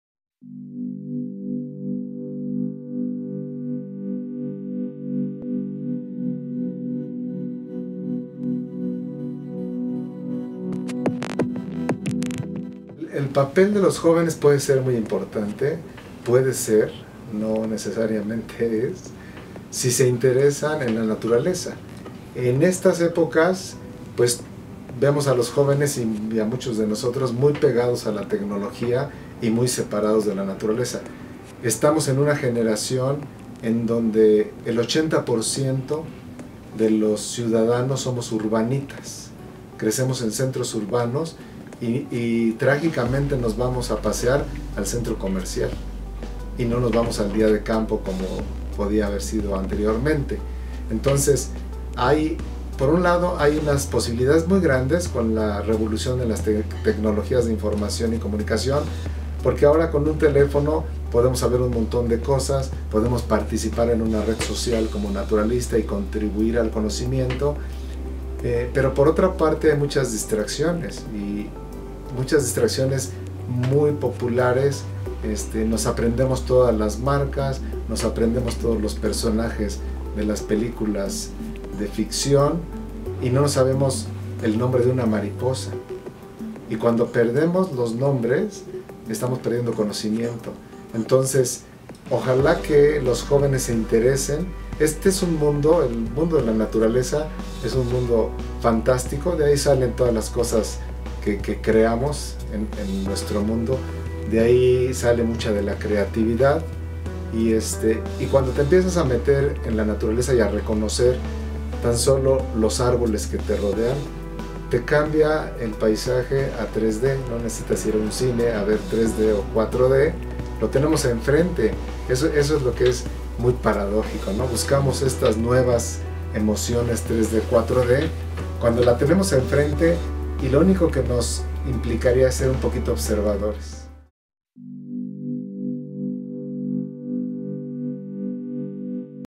Entrevista al Dr. Carlos Galindo de CONABIO_03
Entrevista al Dr. Carlos Galindo con motivo del reconocimiento Universitario Caracol de Plata, XIII Edición y que lleva como tema Conservación de la Biodiversidad.